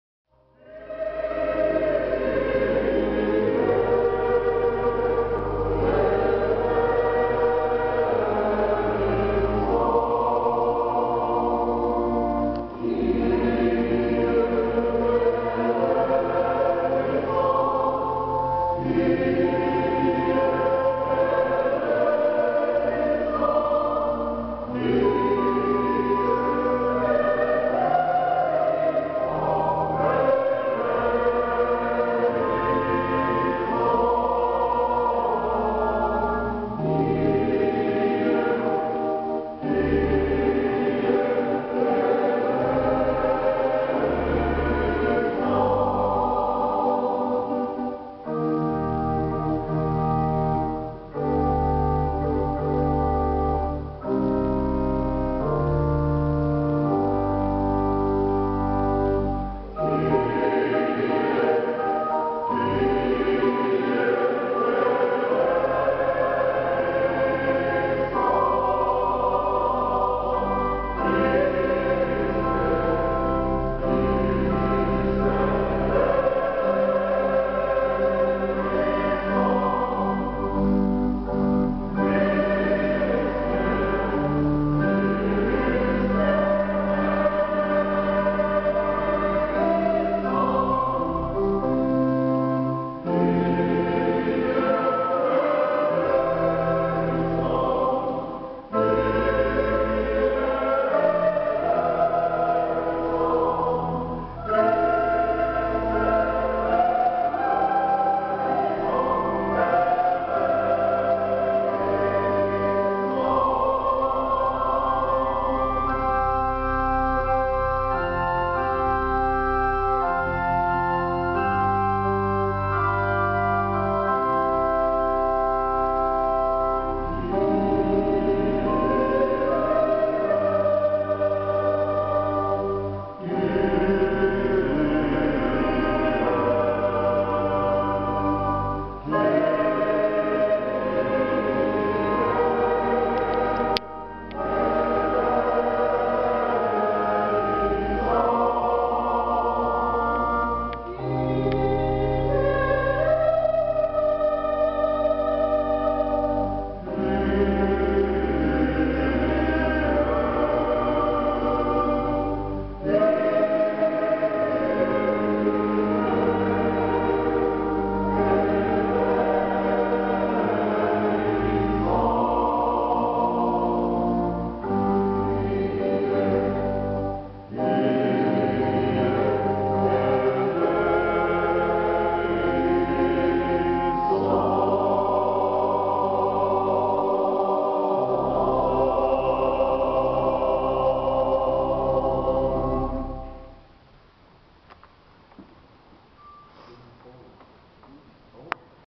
C.C. 25 de Julho - Coros no Estado de S.P. - Mosteiro São Bento - Vinhedo 6
Os dois Coros do C.C. 25 de Julho - parte dos coralistas, em visita ao Mosteiro de São Bento - Capela projetada pelo arquiteto Hans Broos e órgão projetado e construído pelo organista e coralista do Liederkranz, Sr. Georg Jann.
Grupo recebido pelo Frei Paulo. Dia 27 de janeiro de 2012.